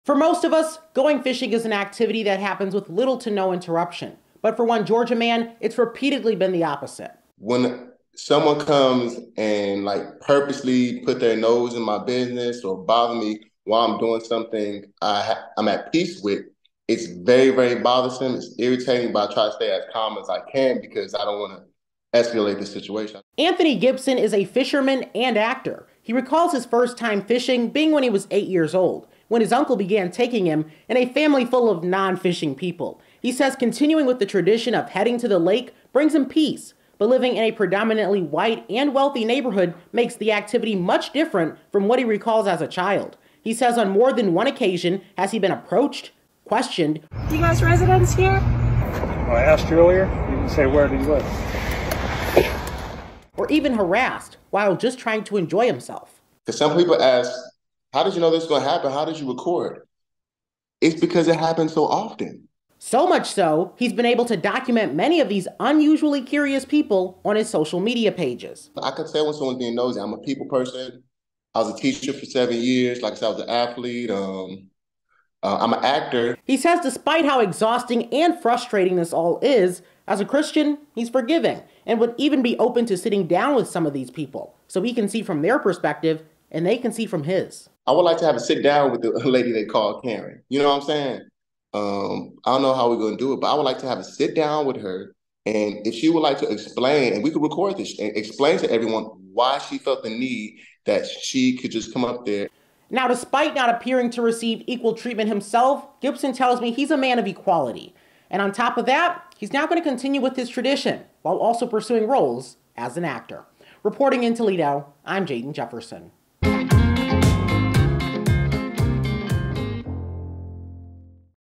Georgia fisherman alleges he's the victim of racial profiling
For most of us, going fishing is an activity that happens with little to no interruption. But for one Georgia man, it's repeatedly been the opposite.